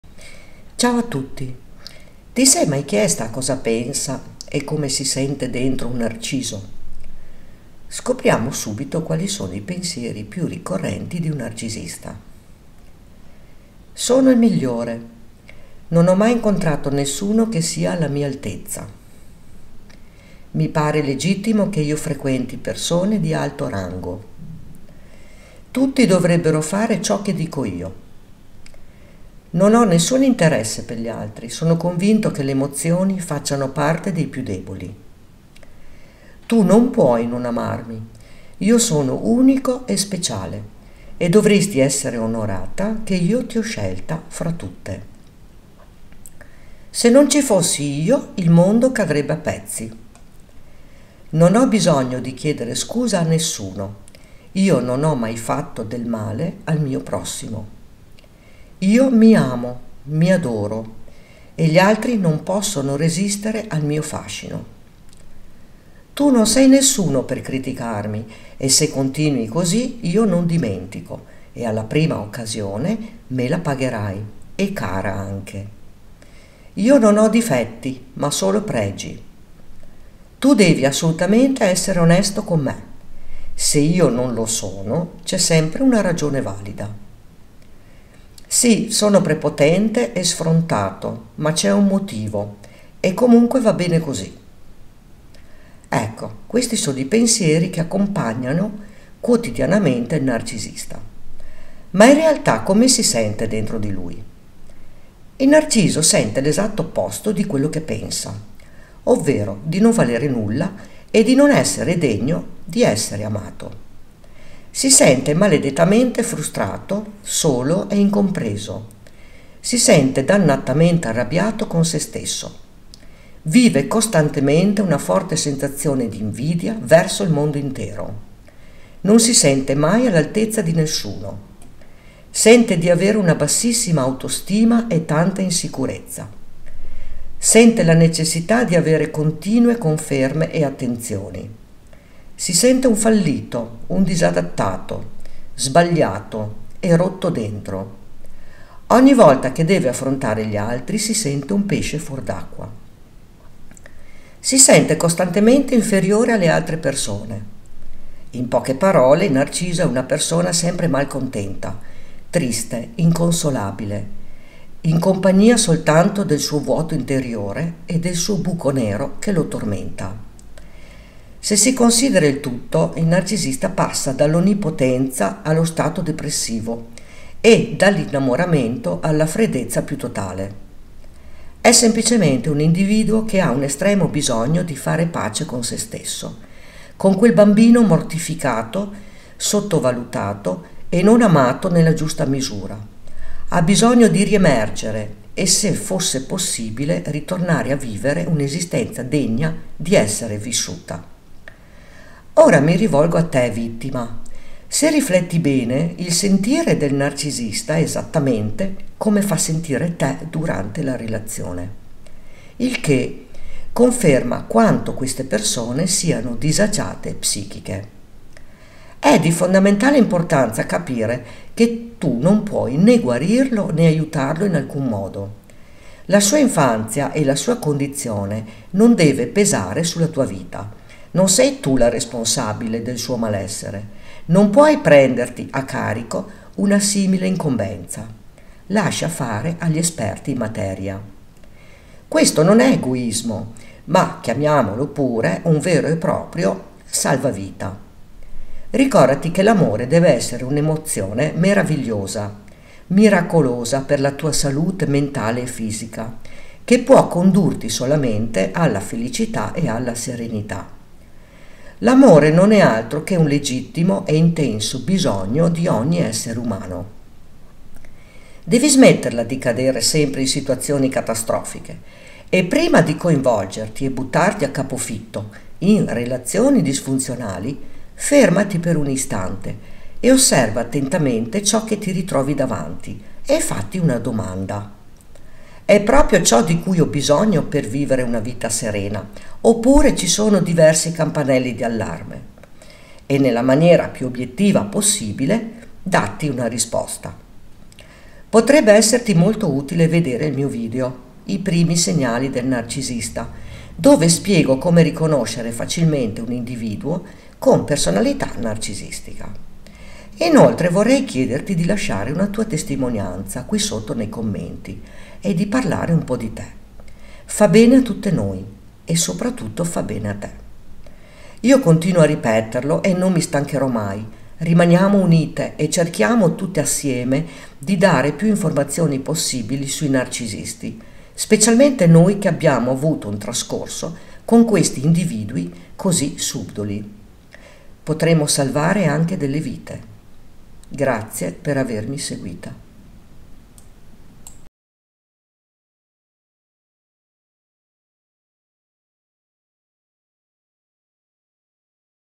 Cosa Pensa e Come Si Sente Dentro il Narcisista?
Ti sei mai chiesta a cosa pensa e come si sente dentro un narciso?
In questo video, scopriremo quali sono i pensieri più ricorrenti e come in realtà, si sente dentro un narcisista.
Inoltre, vorrei chiederti di lasciare una tua testimonianza qui sotto nei commenti e di parlare un po' di te.
Fa bene a tutte noi e soprattutto fa bene a te!